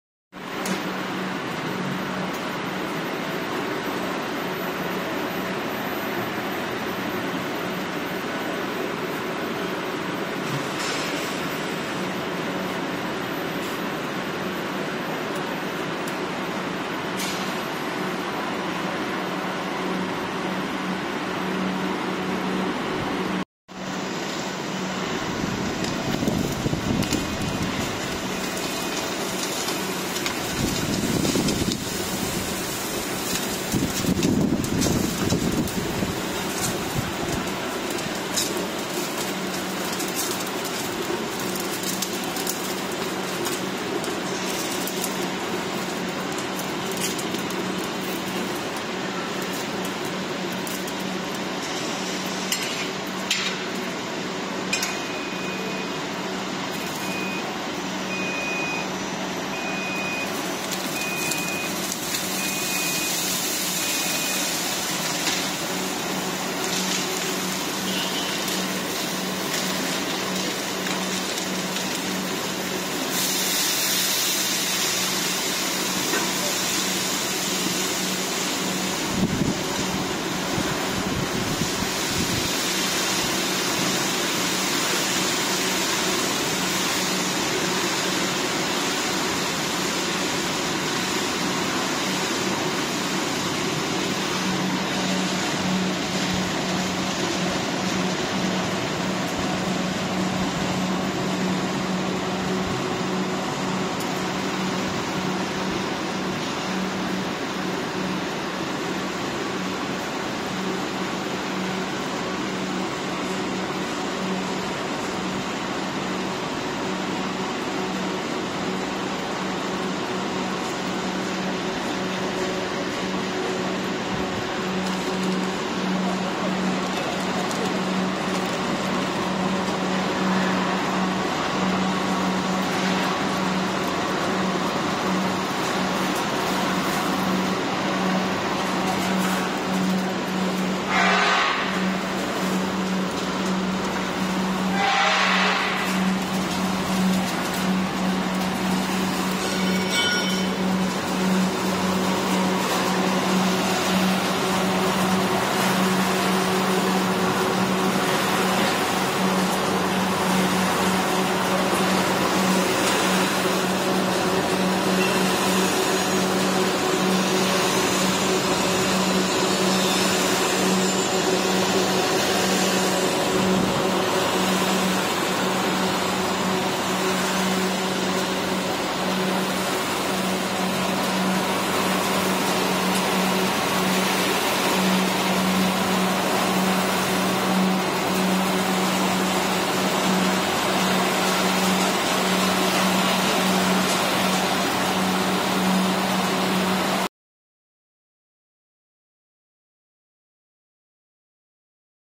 Satisfy Your Snack Cravings: Small-Scale Popcorn Production Line Unveiled!
Step into the world of popcorn production with our mesmerizing small-scale popcorn production line!

Watch as kernels are transformed into the iconic crunchy snack through a seamless process of popping, flavoring, and packaging.

Discover the artistry behind popcorn making and witness the magic unfold right before your eyes.

Join us on this mouthwatering journey and explore the delectable world of popcorn delights!